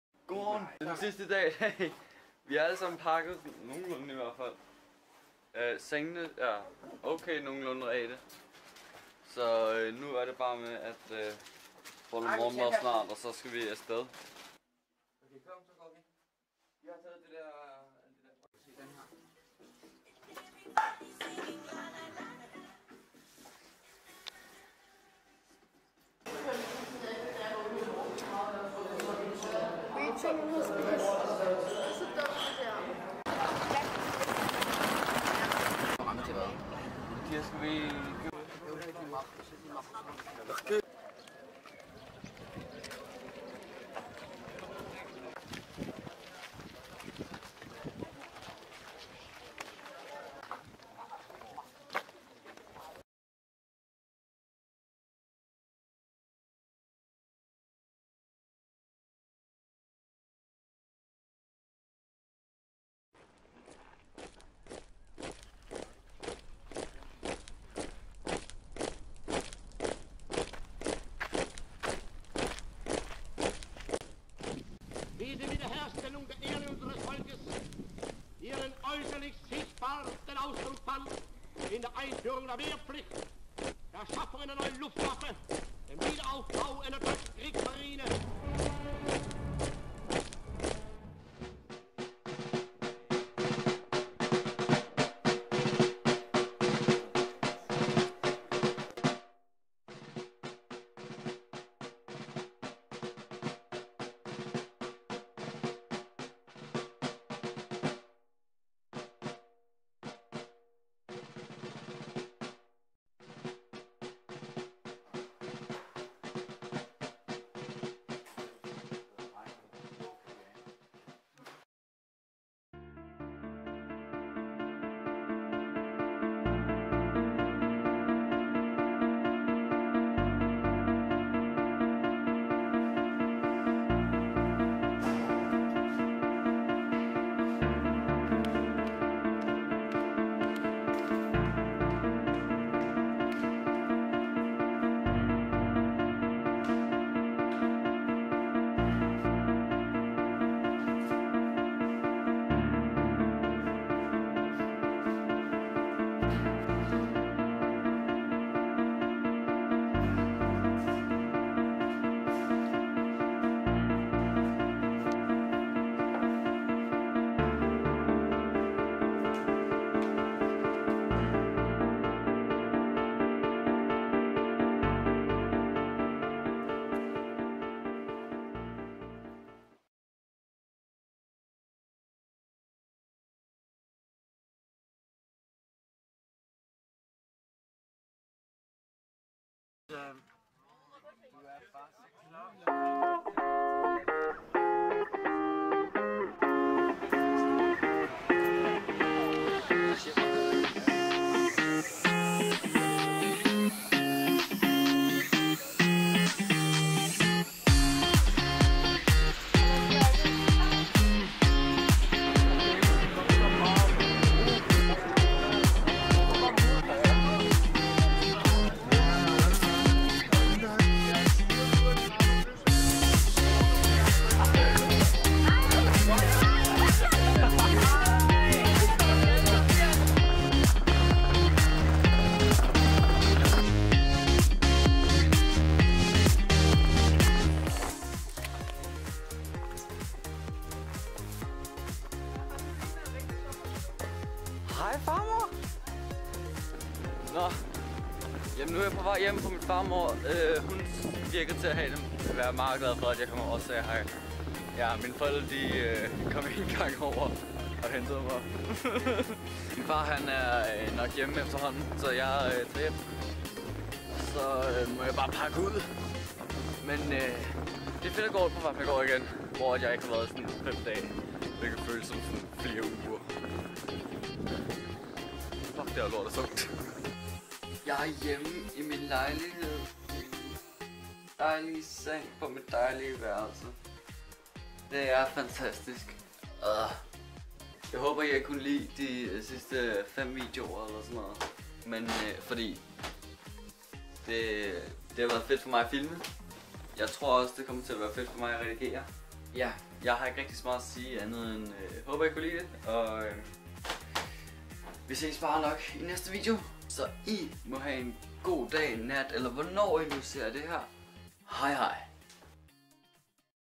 et stop på vejen hjem | Berlin Vlogs
så skal vi hjem men vi tager lige et stop ved Sachsenhausen koncentrationslejr